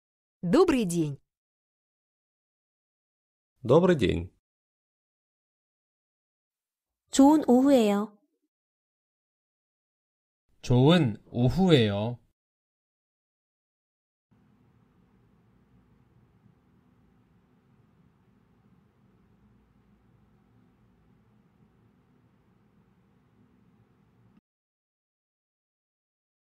러시아어로 "좋은 오후예요." 말하는 법
남자 목소리 :
Добрый день.

여성 목소리 :
Добрый день.

남성 음성 번역 :
좋은 오후예요.

여성 음성 번역 :
좋은 오후예요.